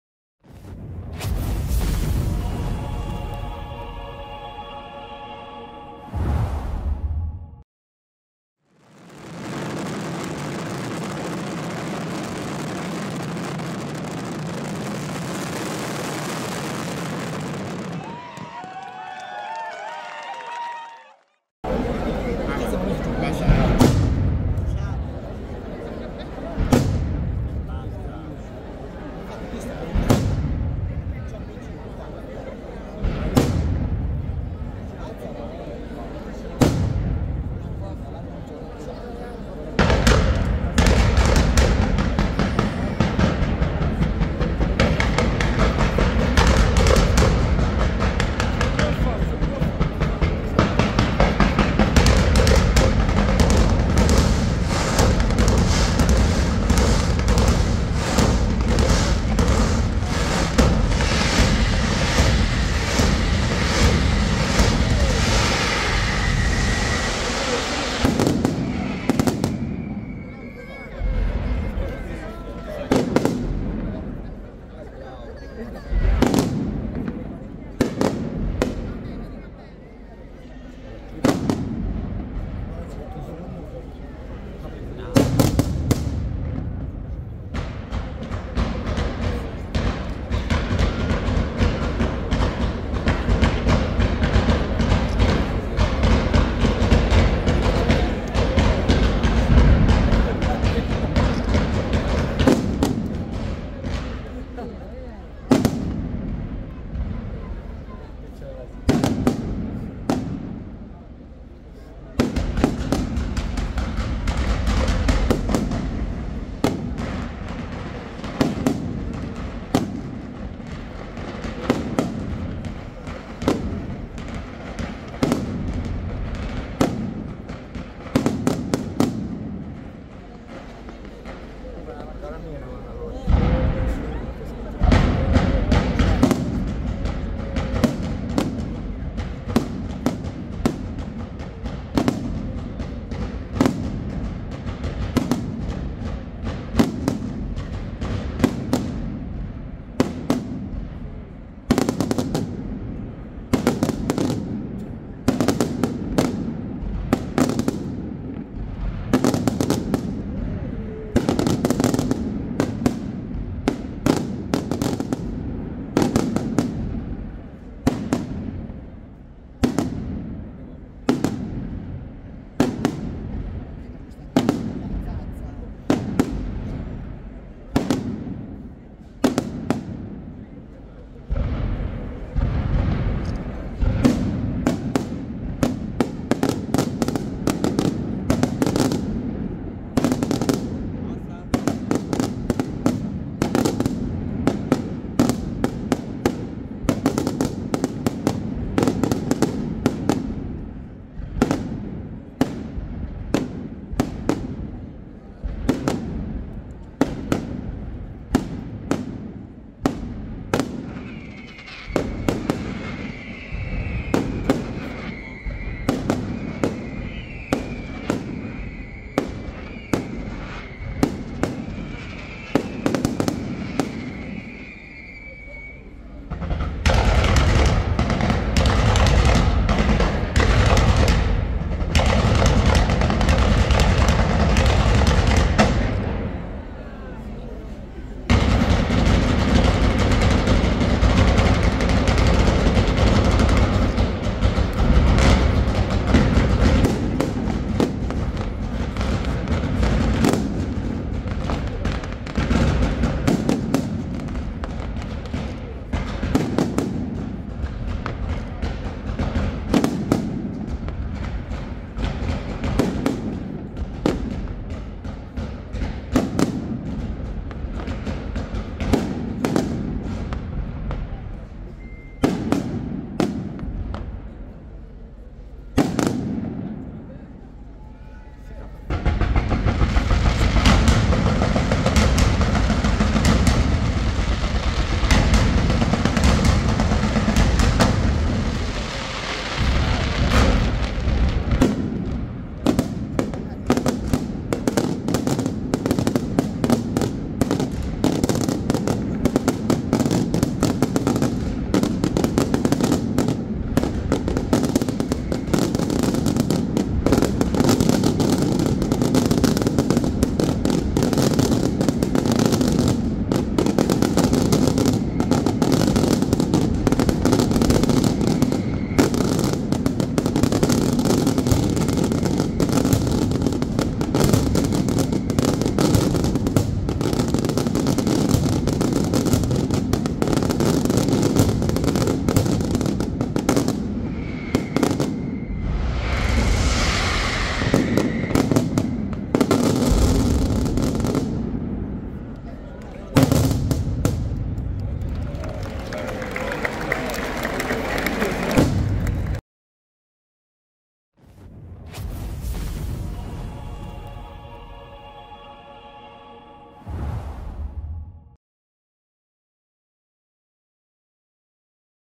4K Palazzolo Acreide (SR) - San Paolo Apostolo 2024 FaSiMa by Spina
Spettacolo pirotecnico aereo serale eseguito dalla Fa.Si.Ma. Fuochi Eventi dei f.lli Spina da Santa Venerina (CT).
Si ringrazia il Comitato Festa San Paolo Apostolo di Palazzolo Acreide (SR) e Vito Agosta per la gentilissima collaborazione.